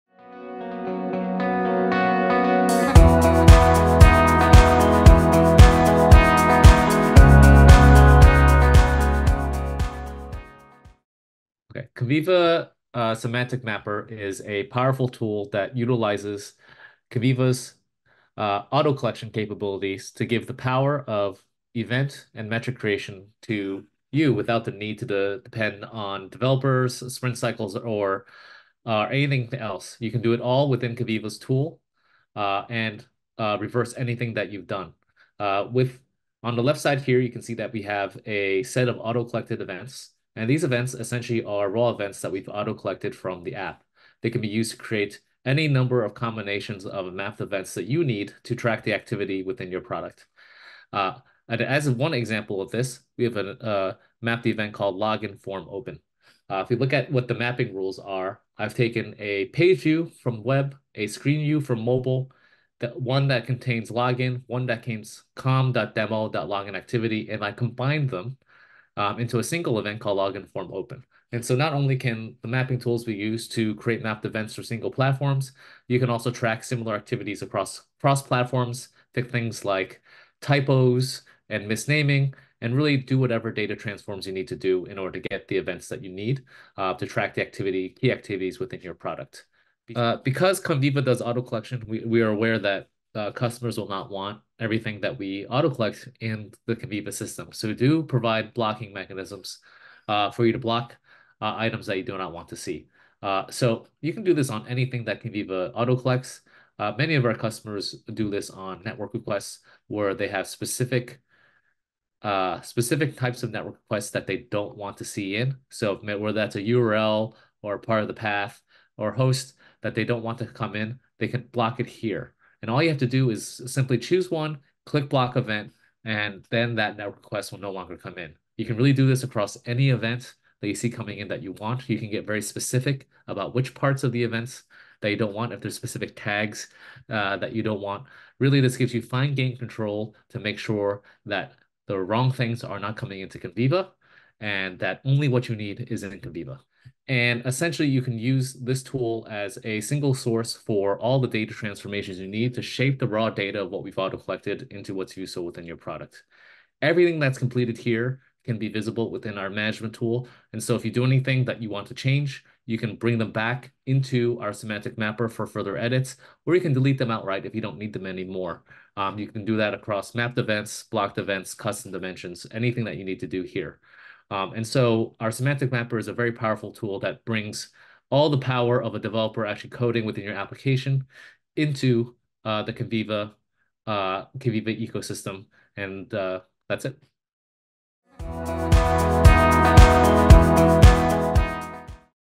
Semantic Mapper - Activate Data that Matters Most
Build and activate only the events and metrics you need. Easily clean, normalize, merge events on the backend, and connect data points from multiple sources.